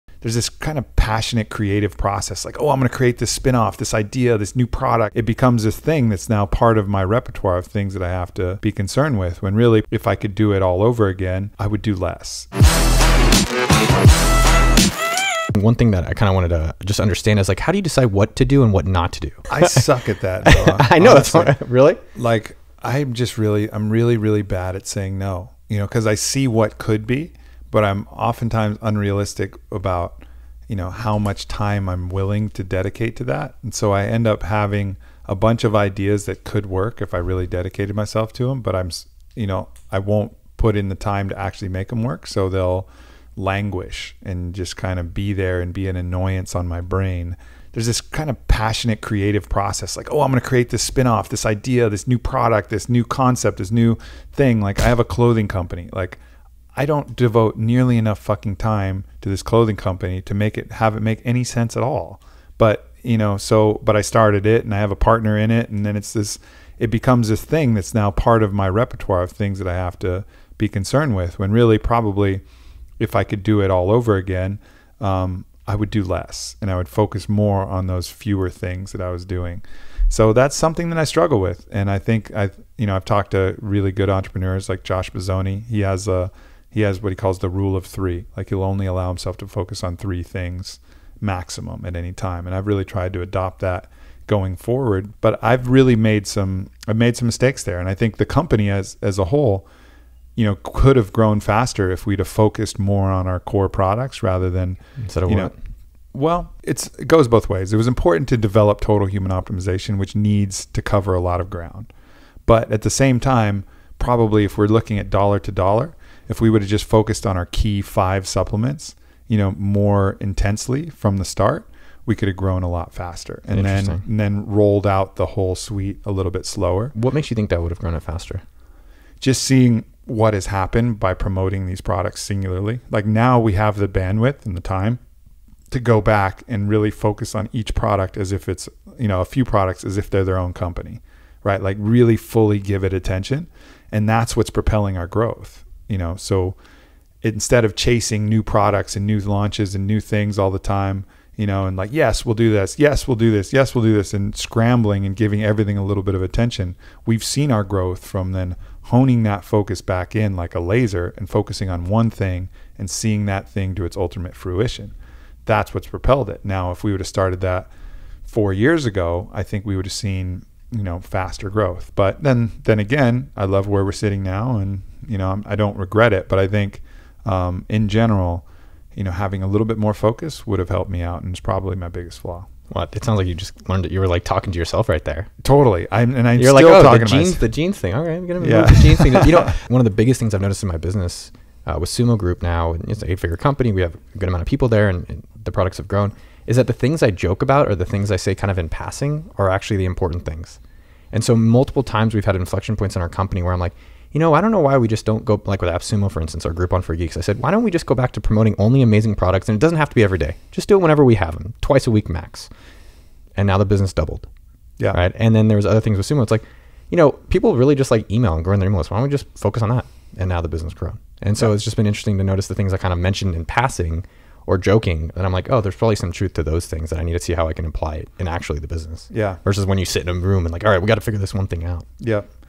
The BIGGEST Regret of an 8-Figure CEO and Founder
The biggest regret for most people is, "I wish I spent more time doing something else."

Sound familiar?
* "I wish I spent more time starting my own company instead of working a 9-5"
* "I wish I spent more timing doing the things I love"
* "I wish I had followed my passions instead of what I felt I should do"

Entrepreneurs feel this regret, too.

Even the biggest names you see on the web — like Aubrey Marcus, the CEO and founder of Onnit — wish they had done things differently.

The truth is, understanding WHERE to put your time can be the difference between living a life of success... or living a life full of regrets.

In this video, learn as Aubrey shares his biggest regret, his strategies for focus, and how the "Rule of 3" can help you improve your own focus.